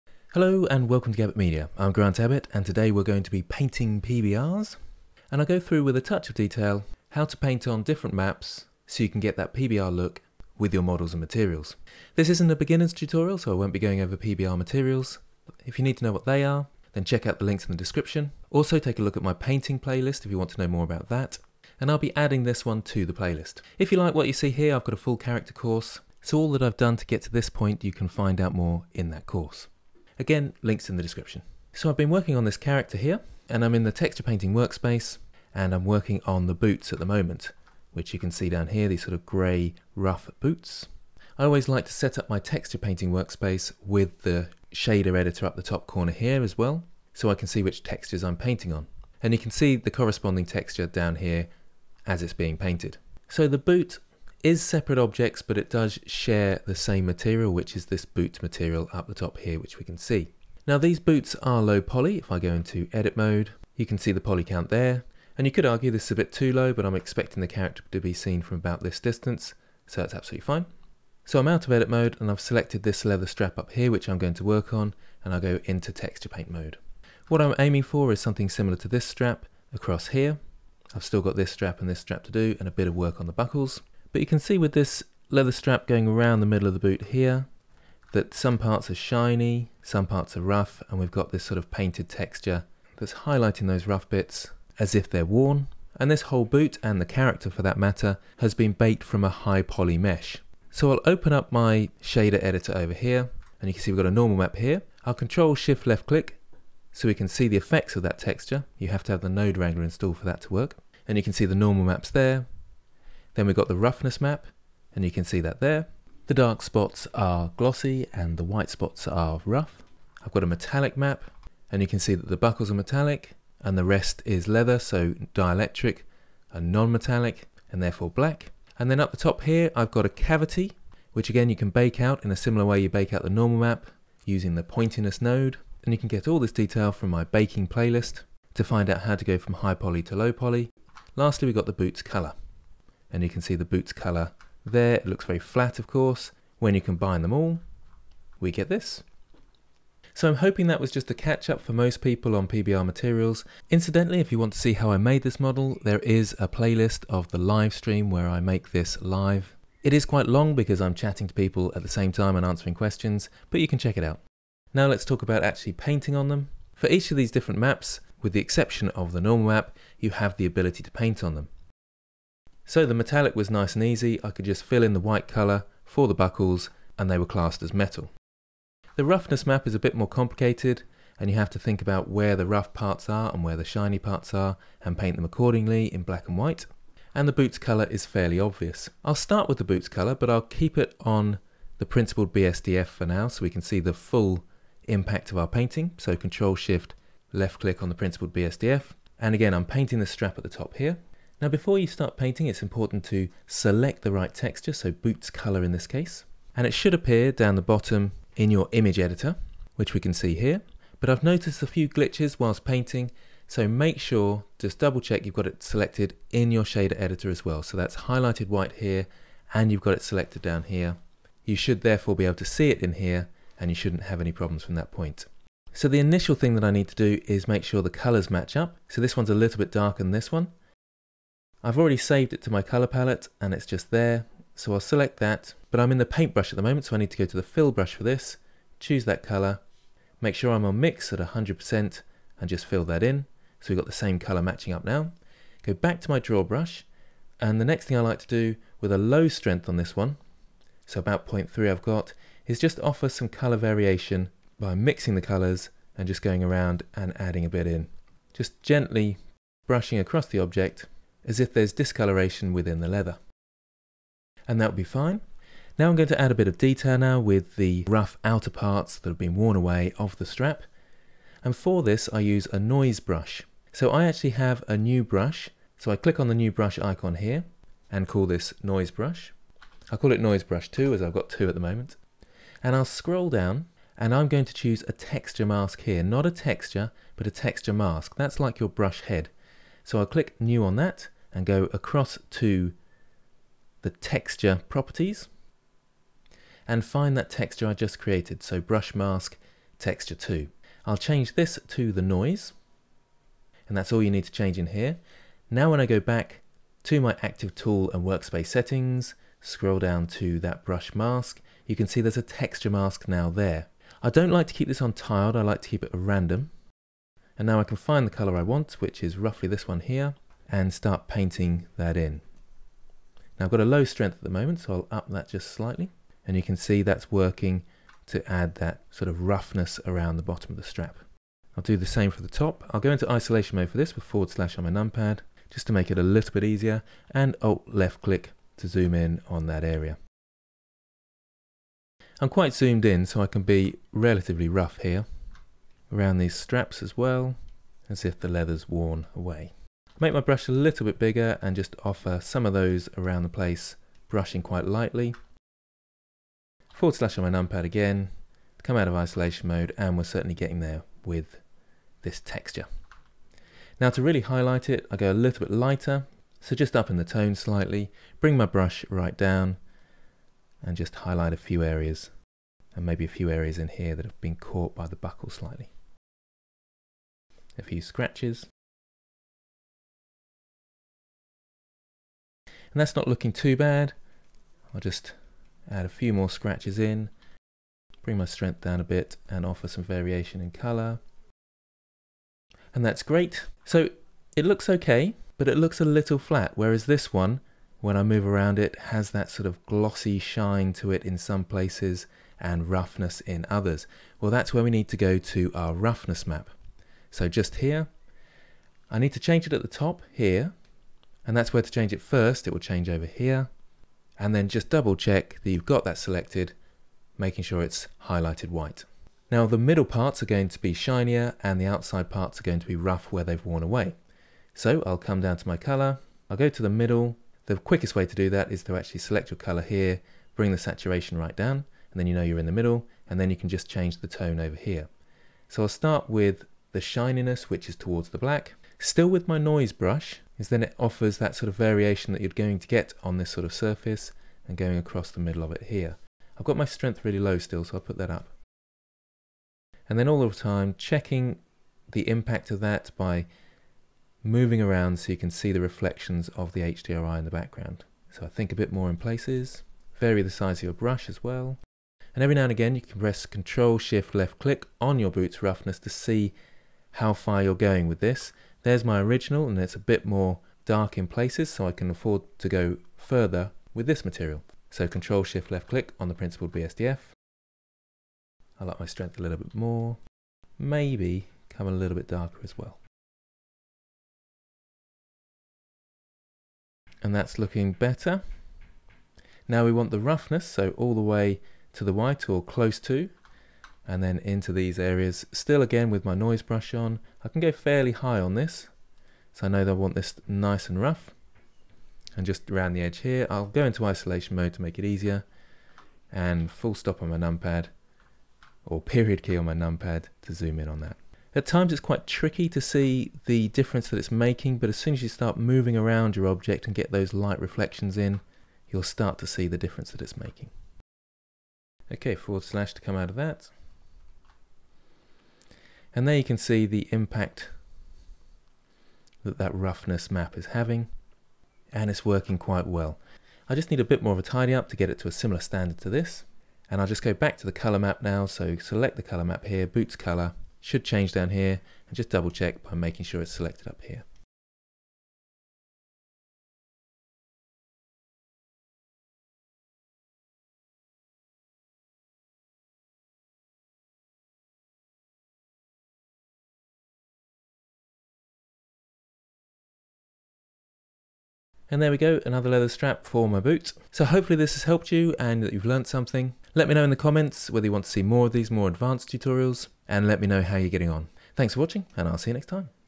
Painting PBR Materials - Guide & Demonstration - Intermediate/Advanced
This is a basic guide and demonstration of painting PBR materials. I believe there are addons for doing this which i will probably take a look at in the future also.

I'll be texture painting the boots of a character i have been working on.

you can see more of the character creation by watching the live stream playlist (warning its very slow pace answering questions etc.)

*Cheapest + FAVOURITE:*
Veikk a15

Display Tablets
*My Favourite*
VEIKK VK1560

Cintique 22”HD
Amazon UK

UK pro 13
UK pro 16